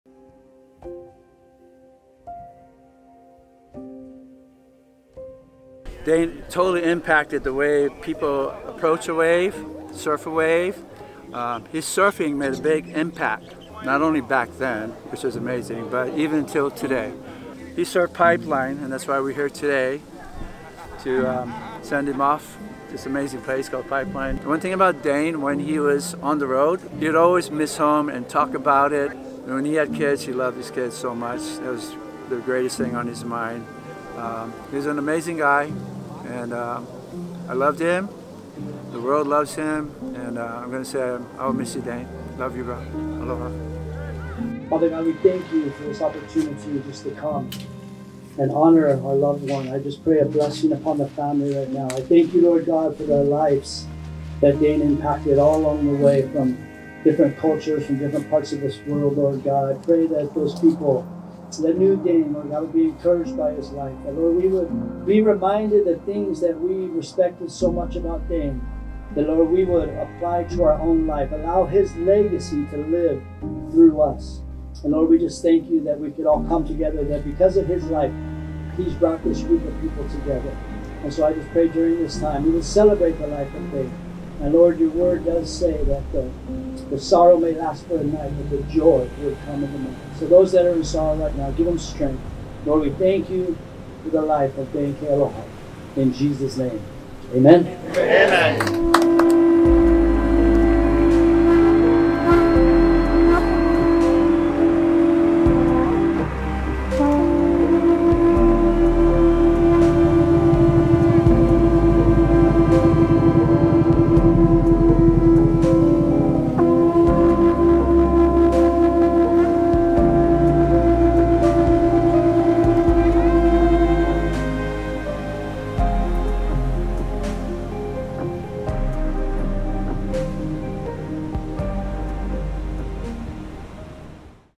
Dane Kealoha Paddle Out - Sunday, July 2nd 2023
This past weekend friends, family, & loved ones of the renowned Hawaiian surfer, Dane Kealoha, paddled out at Pipeline to spread his ashes in the ocean. Dane was a true Hawaiian waterman who changed surfing forever through his powerful style. He will always be remembered for his love of the ocean, surfing, & his children. On this beautiful day, surfers were greeted by a pod of dolphins upon entering the water. A sure sign to all that Dane was watching. We love and miss you Dane Kealoha. A hui hou kakou. Aloha.